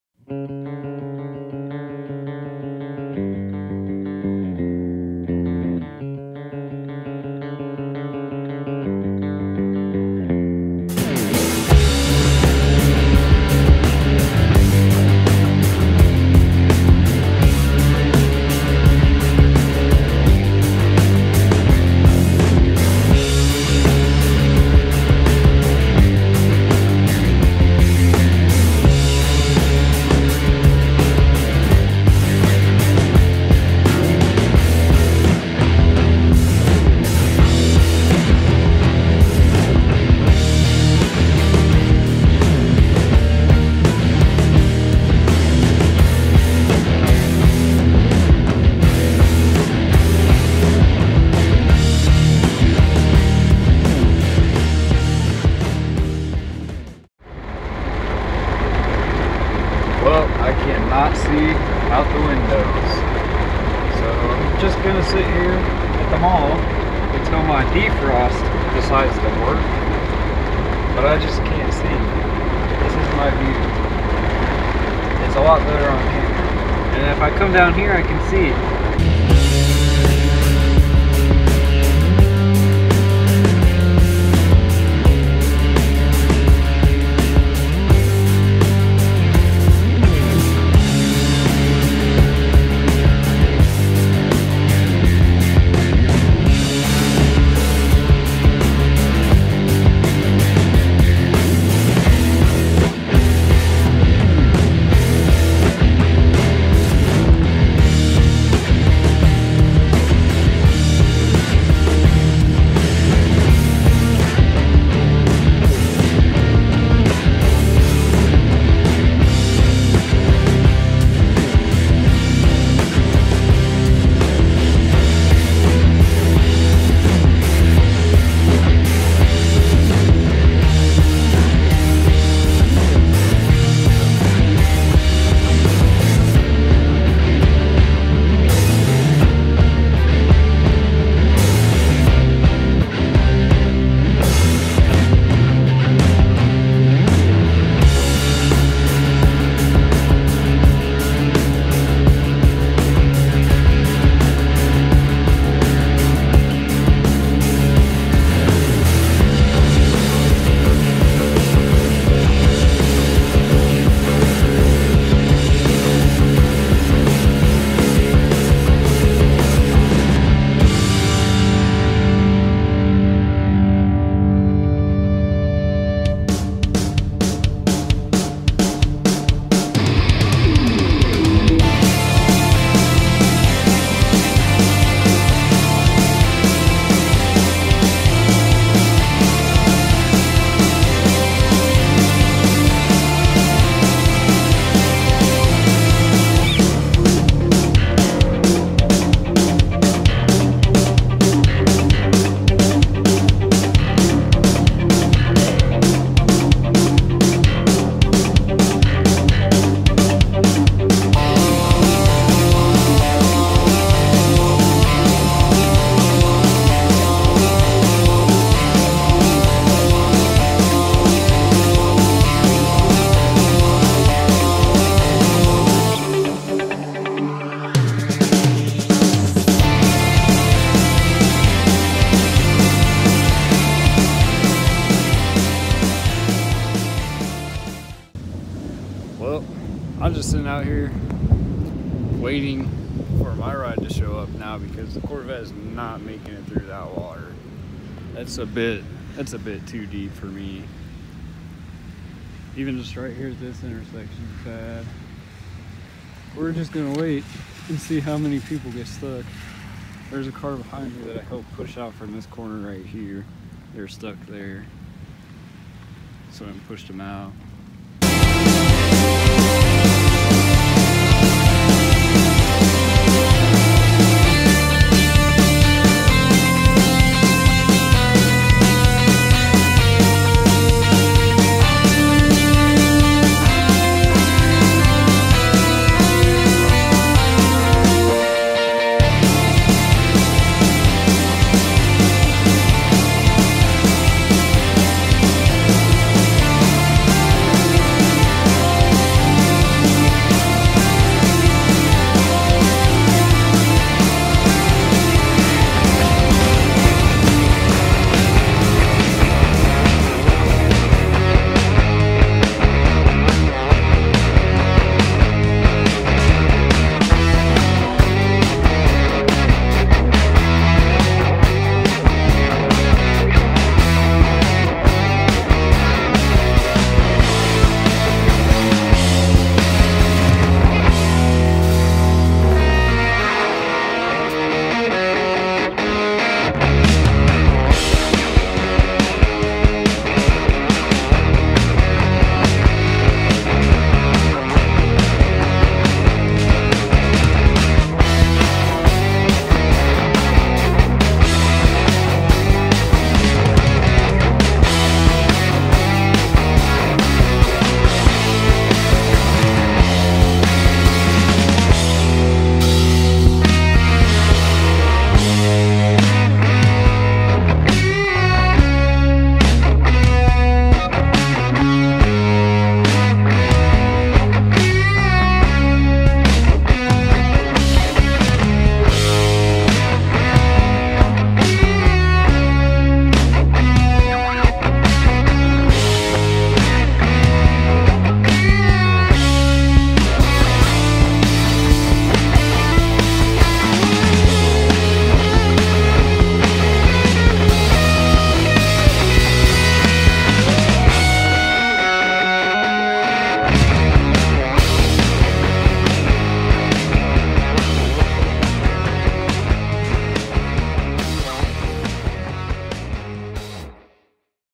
Corvette Flooded by Storm - Flash Flooding
I thought I'd be able to get home before the streets flooded... I was WRONG! This is video from my dash cam, and a clip from my phone about the adventure of getting home in a Corvette with flooded streets.

Discounts:
Use Code ArcticVette

32gb SD Card (2 pack)